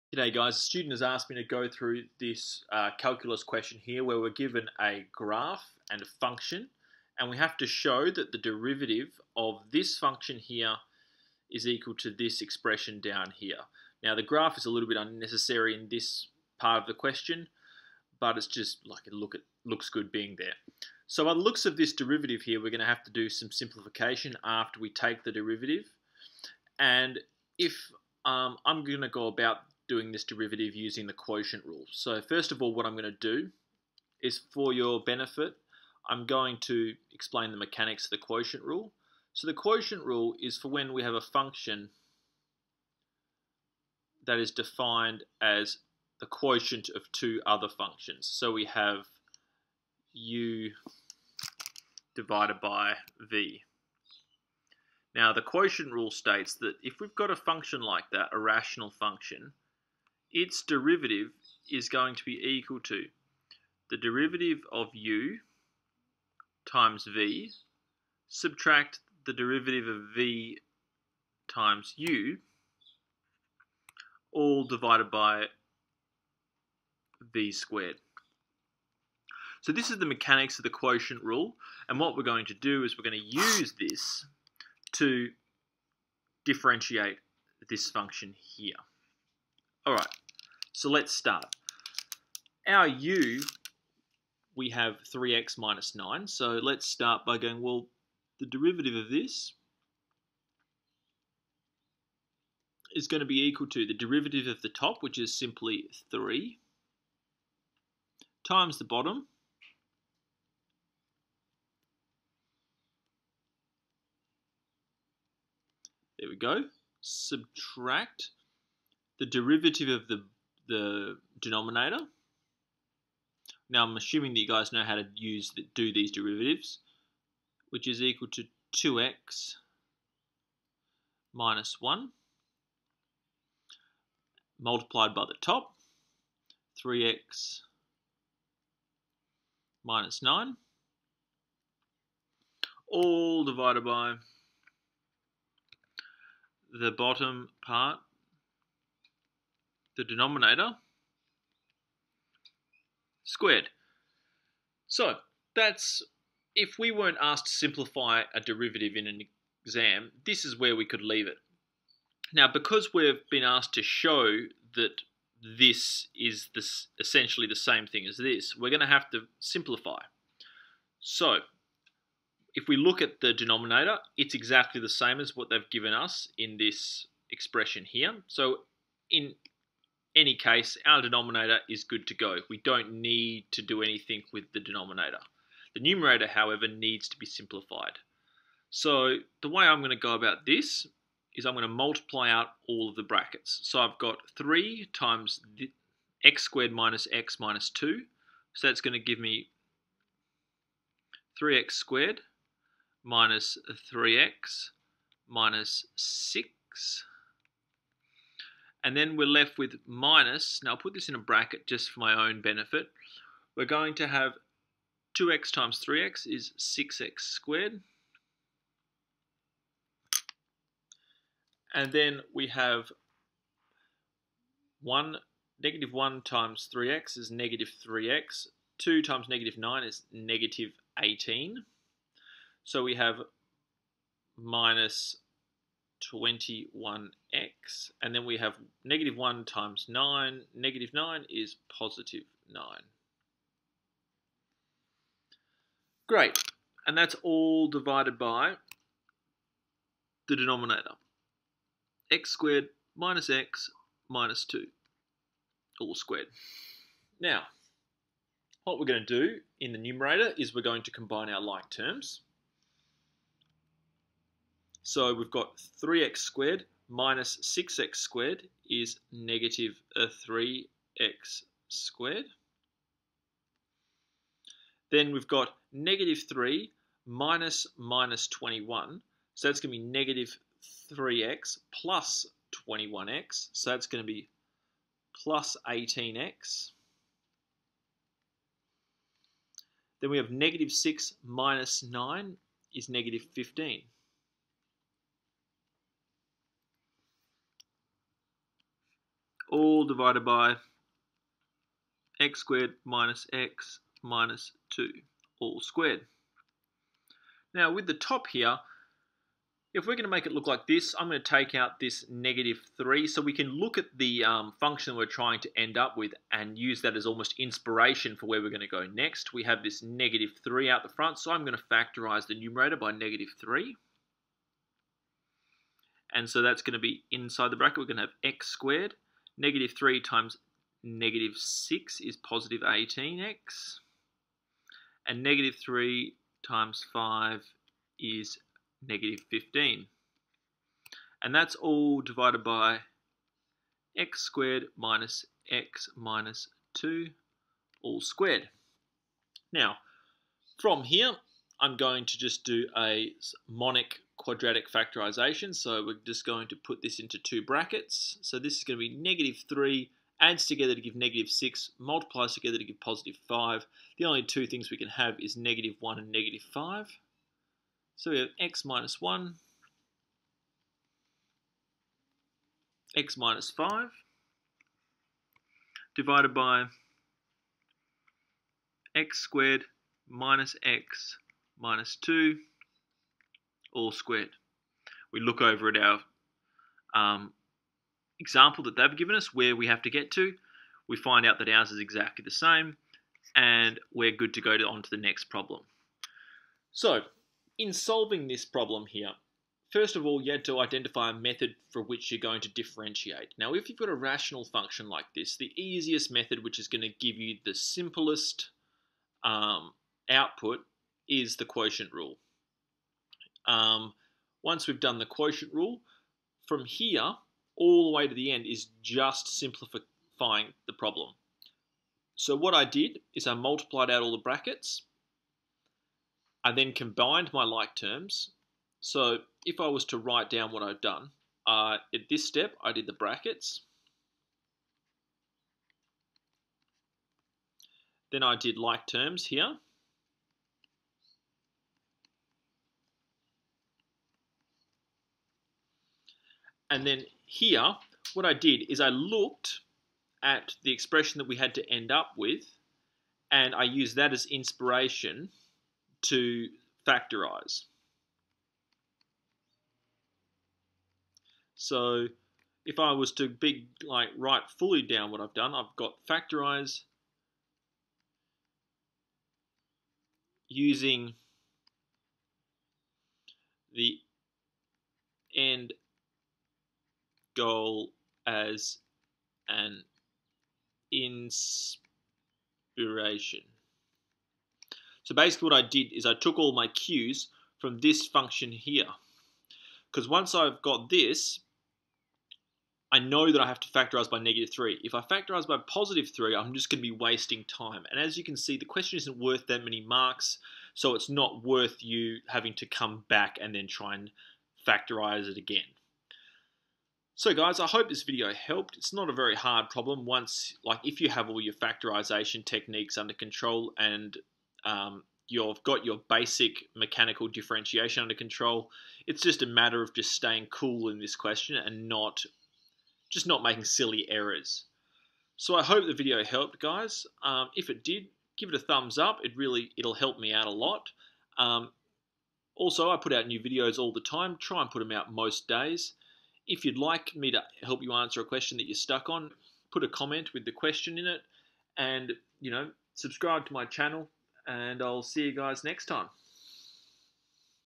Differentiation - Quotient Rule: Exam Example
In this video we are asked to use differentiation to verify whether the gradient function of a curve is as written. Because of the rational nature of the function the easiest way to differentiate it is by using the quotient rule.

This example question was pulled from the 2015 Mathematical Methods practice exam.

Cheers